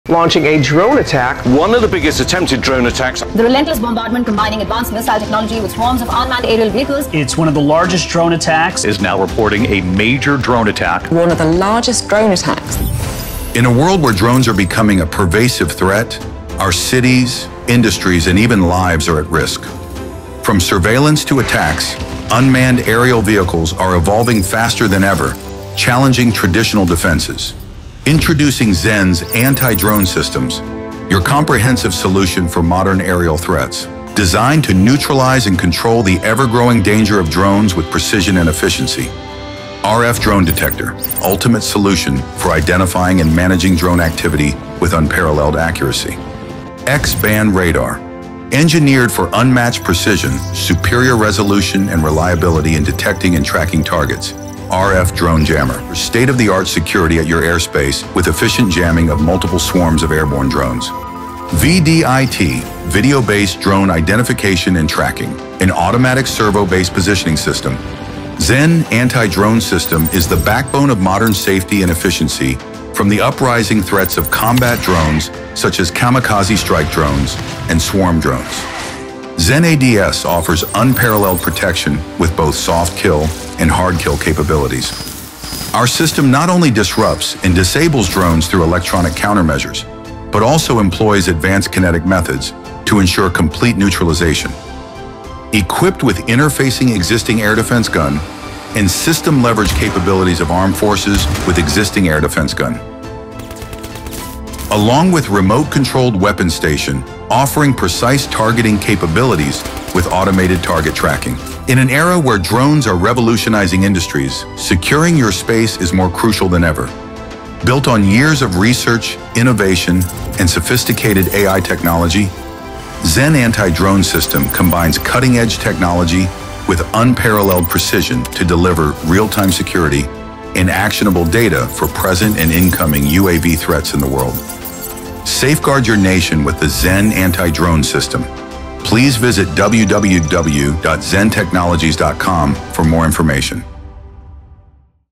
Zen Anti Drone System - Counter Unmanned Aerial System (CUAS)
Anti Drone System from Zen Technologies works on drone detection, Classification and tracking on passive surveillance, camera sensors and neutralization of the threat through jamming the Drone communication.

Zen Anti Drone System (CUAS)/Counter Unmanned Aerial System is a multi-layer multi sensor Architecture aimed at providing comprehensive security against drone attacks.